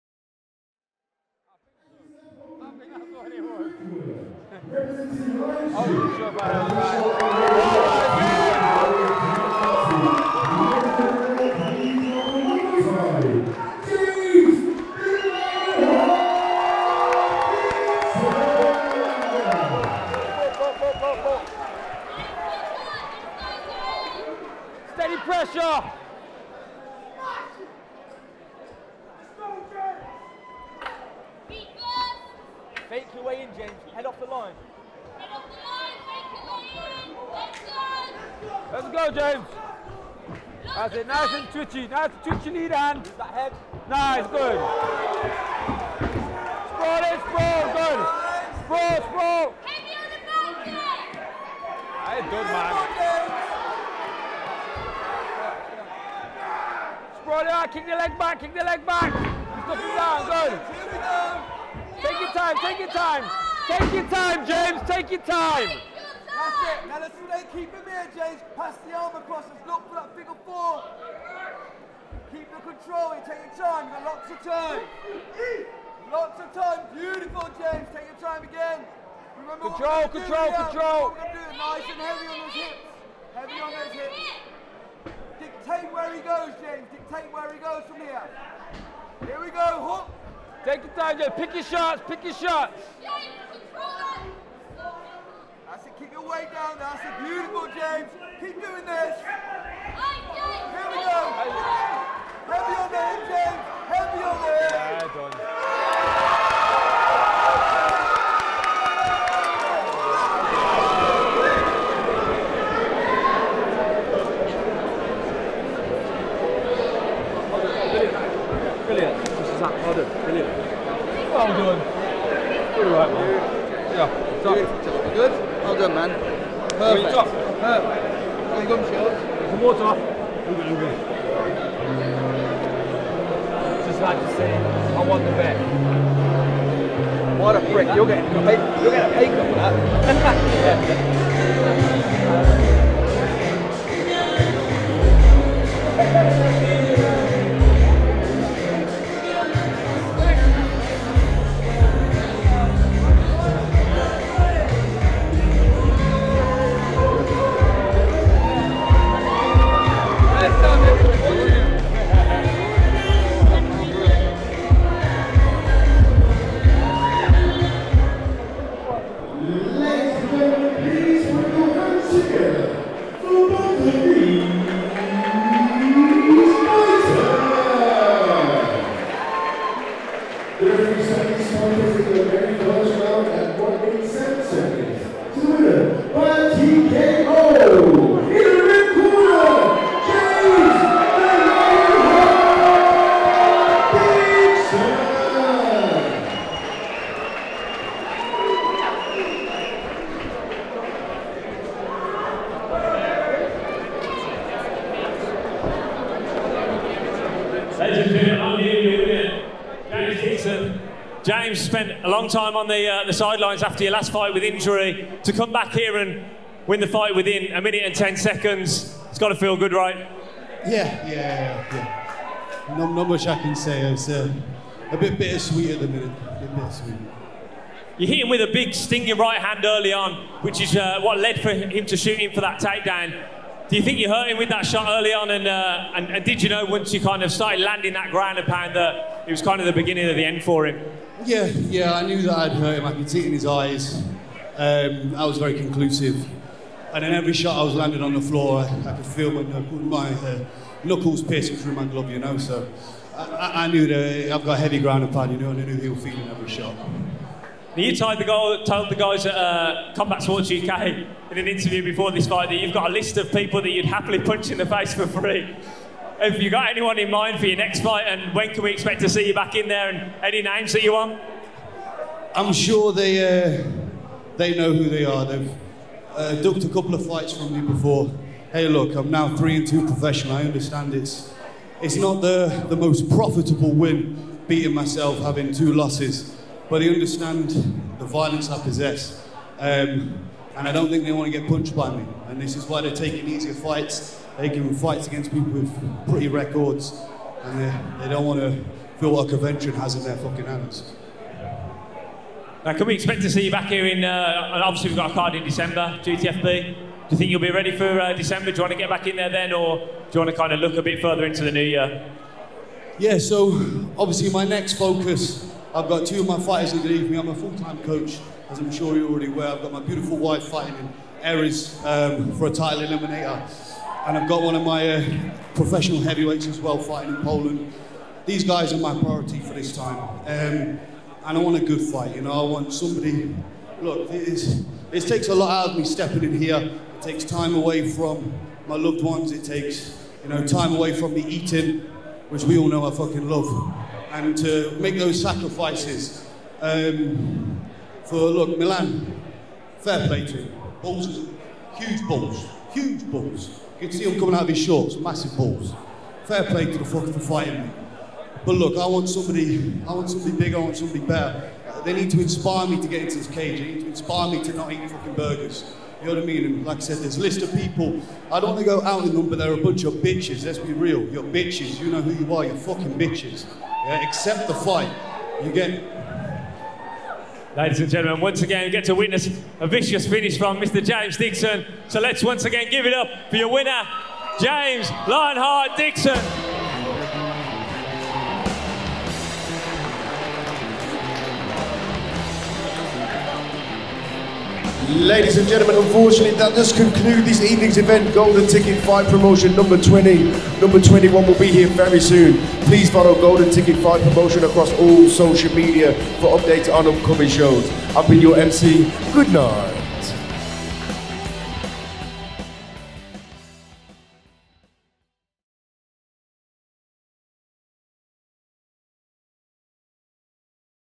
GTFP 20 - James Dixon vs Milan Silva
James Dixon made his 5th walk to the octagon as a professional on 3rd September 2022 to face off against his opponent, Milan Silva.

Within seconds on the fight beginning, James was able to dictate the pace of the fight with his power and aggression, controlling what happened on the feet, against the cage, and ultimately on the ground where the fight found it's end in under 2 minutes of the first round.

Strictly not for use without license or authorisation from the owner (ME!).

Get in touch for further information.